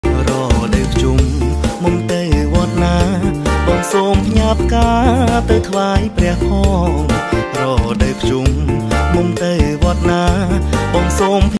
រីករាយពីធីបុណ្យភ្ជុំបិណ្ឌ
ក្រុមហ៊ុនយើងខ្ញុំនឹងធ្វើការឈប់សម្រាករយៈពេល ៣ថ្ងៃ គឺ៖
ថ្ងៃទី ២៤-២៥-២៦ ខែកញ្ញា ឆ្នាំ២០២២​ នឹងបើកដំណើរការវិញ នៅថ្ងៃទី ២៧ ខែកញ្ញា ឆ្នាំ២០២២៕
សូមអរគុណ !.